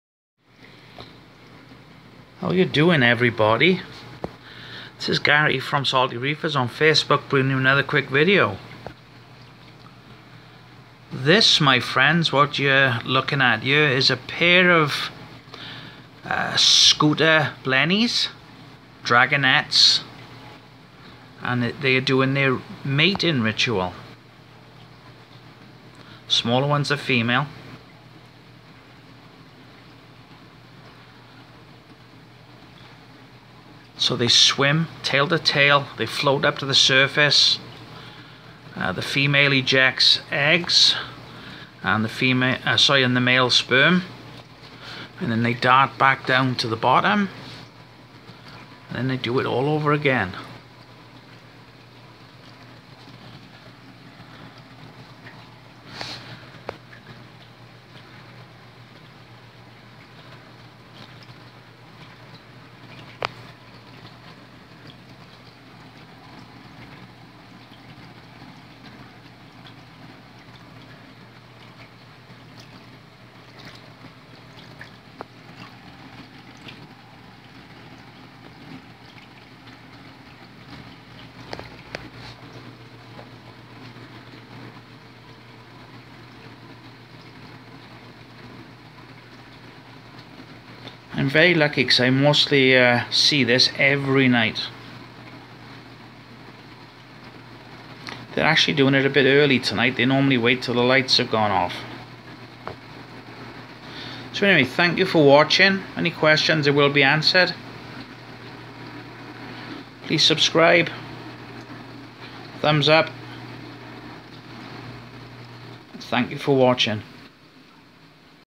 Scooter Blennies mating Ritual Dance
My Mr and Mrs Scooter Blennies [Dragonettes] Synchiropus Ocellatus. They are doing there almost nightly Mating Ritual.